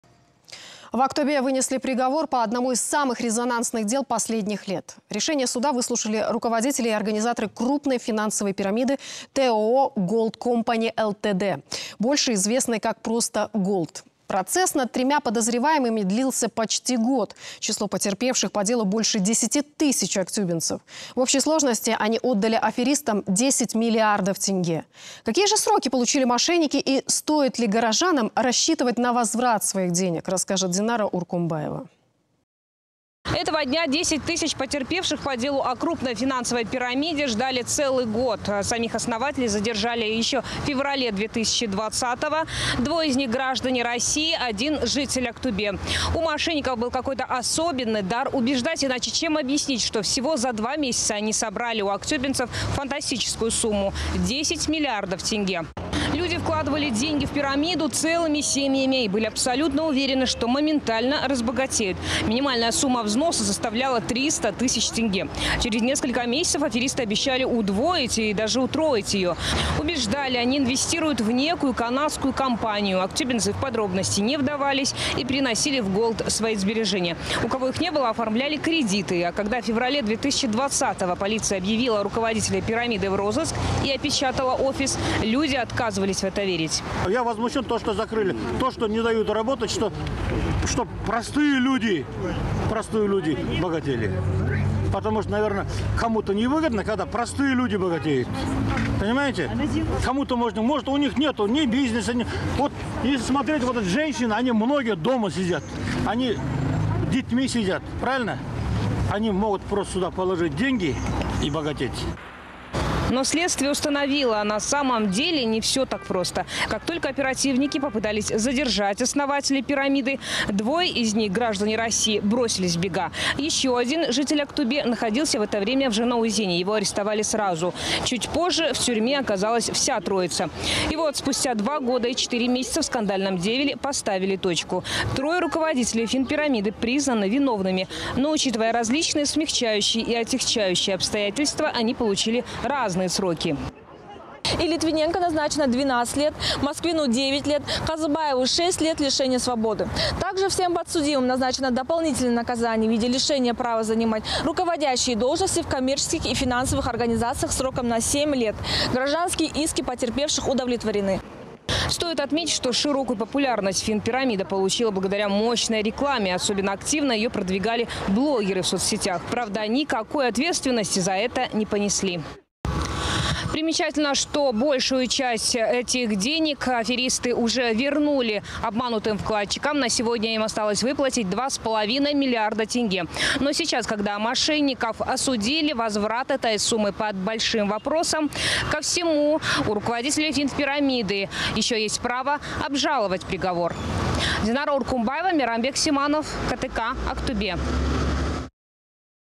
От 6 до 12 лет получили руководители финпирамиды «Голд»
В Актобе вынесли приговор по одному из самых резонансных дел последних лет. Решение суда выслушали руководители и организаторы крупной финансовой пирамиды ТОО GOLD Company LTD, больше известной как просто «Голд». Процесс над тремя подозреваемыми длился почти год. Число потерпевших по делу – больше 10 тысяч актюбинцев. В общей сложности они отдали аферистам 10 миллиардов тенге. Какие же сроки получили мошенники и стоит ли горожанам рассчитывать на возврат денег?